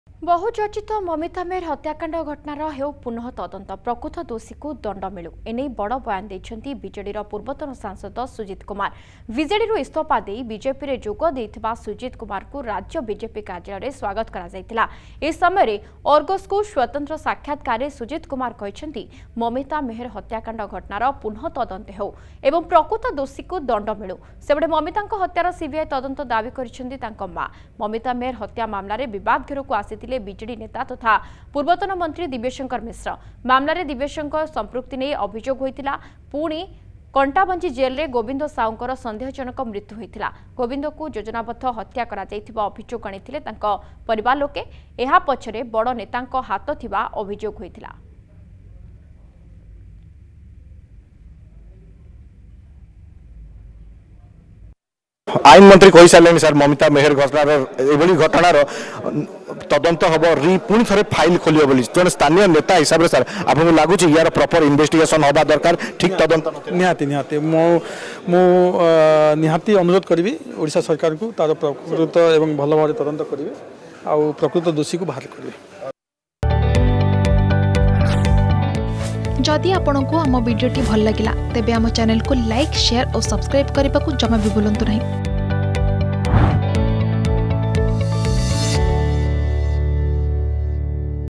`ମମିତା ହତ୍ୟା ମାମଲାର ହେଉ ପୁନଃ ତଦନ୍ତ' || Mamita Death Case Investigation
`ମମିତା ହତ୍ୟା ମାମଲାର ହେଉ ପୁନଃ ତଦନ୍ତ' । ପୂର୍ବତନ ସାଂସଦ ସୁଜିତ୍ କୁମାରଙ୍କ ବଡ଼ ବୟାନ ।